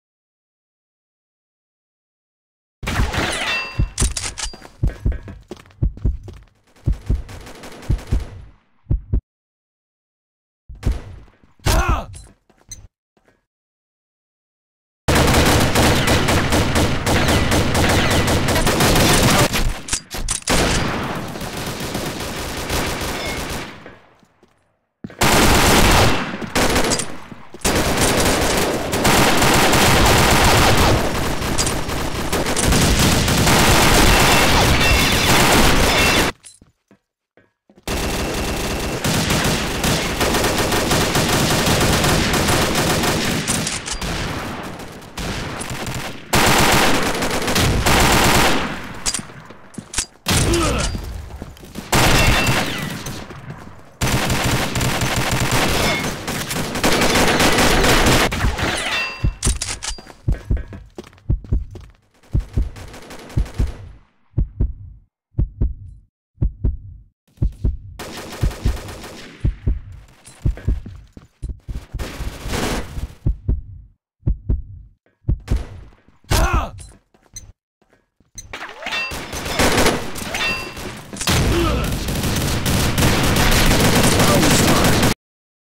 Reupload: Pointless vid
Reupload because of random blackness at the end.

Trying to record a hacker and some MAJ calls me a camper and QQer